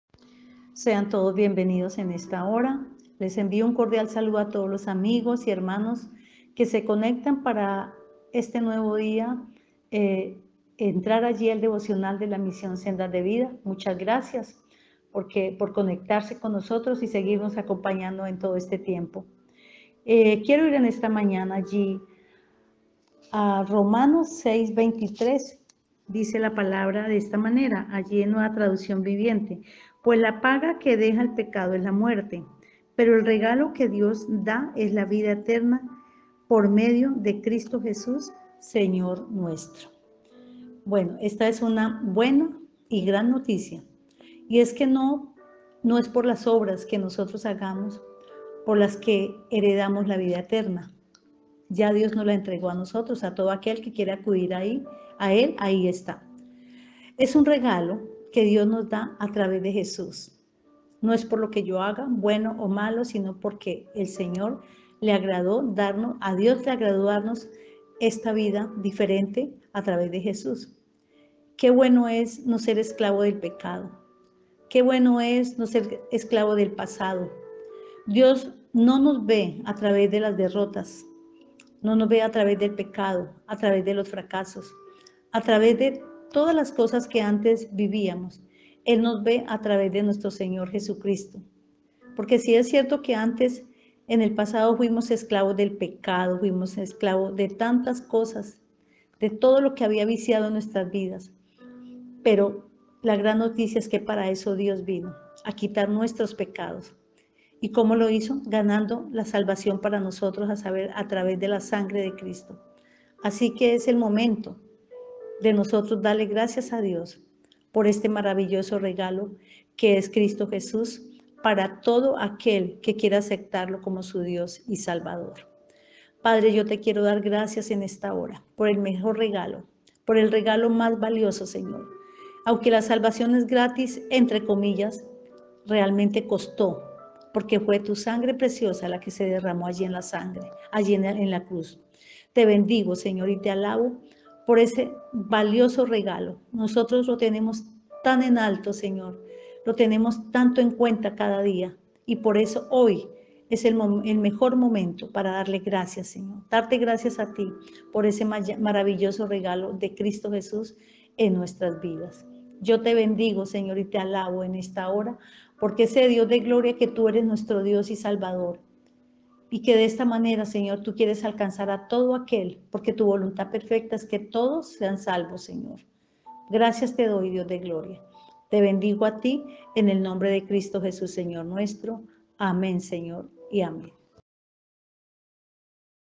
Devocional Pastora Elcy - 24/08/22 - Romanos 6:23
Demos gracias a Dios que ya no somos esclavos del temor, ni del pecado, siendo el pecado una carga que sólo Cristo pudo llevar.